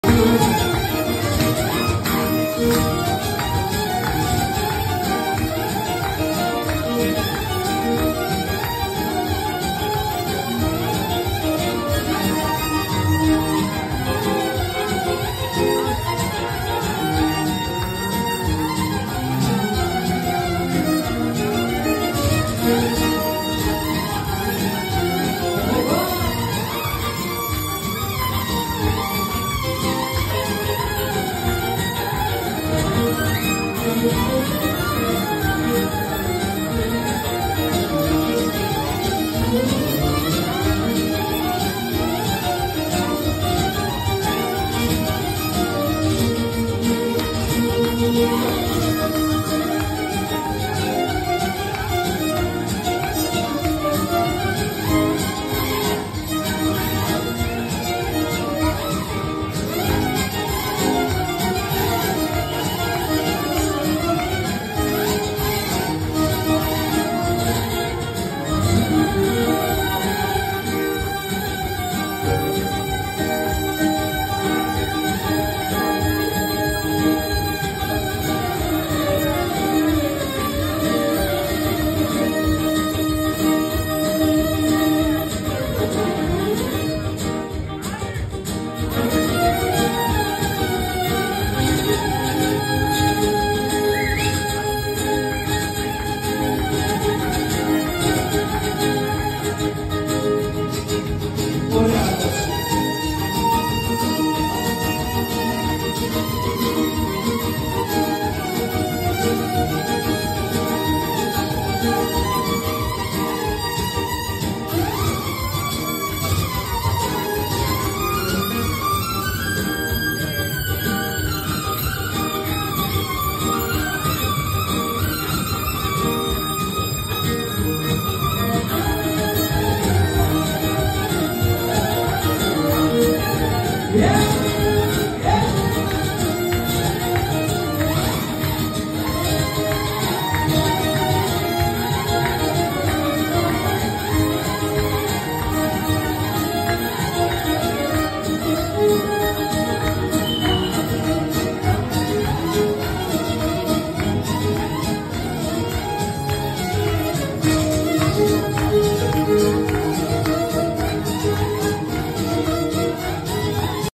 Στέλιος Κλουβάτος & Αριστείδης Βερύκοκκος - Μπάλος Μι Ματζόρε - The passion, στο Γαλάτσι
Ένα μικρό απόσπασμα από το γλέντι μας στο Γαλάτσι στο Passion!
Βιολιά: Στέλιος Κλουβάτος και Αριστείδης Βερύκοκκος
Λαούτο: Βασίλης Τζαννίνης
Κιθάρα: Μιχάλης Φωτιάς
Τύμπανα: Γιάννης Μπουρνέλης